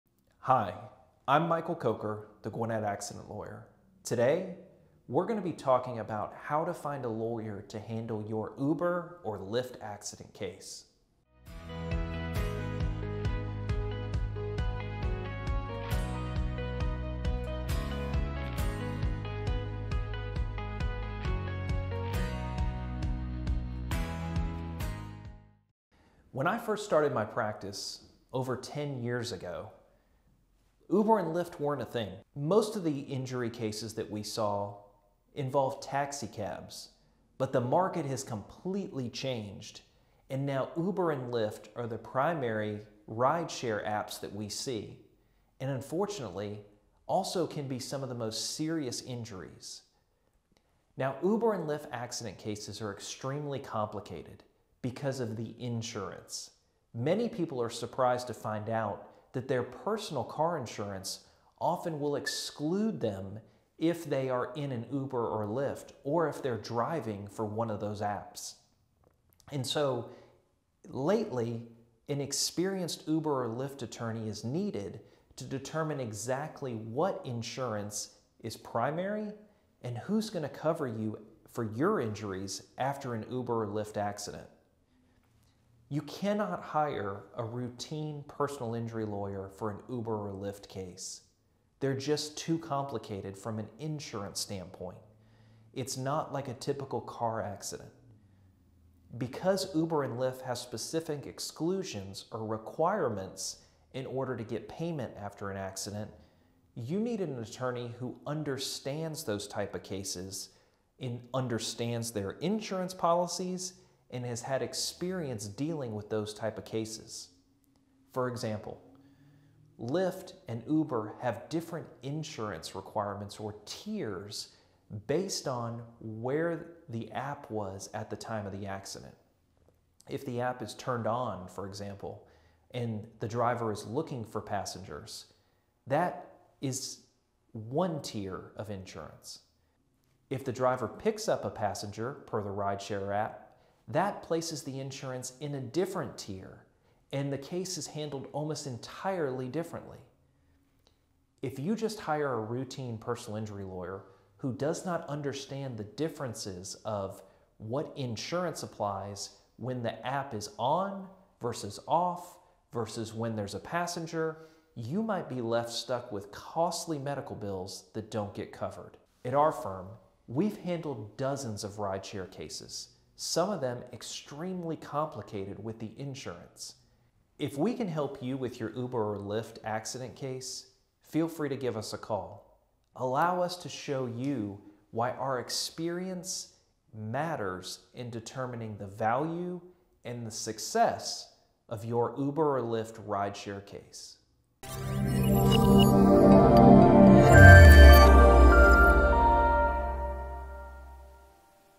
How to Find Your Uber or Lyft Accident Lawyer
Lyft and Uber Accidents can be scary and costly situations. The medical bills can add up and you may not know where to start to get the money you deserve. Here is how to find your Lyft or Uber accident lawyer.

0:00 intro
0:50 Complexities of Uber and Lyft Accidents
1:10 Must Hire an Experienced Lawyer
1:57 Different Types of Insurance Requirements
2:56 More Information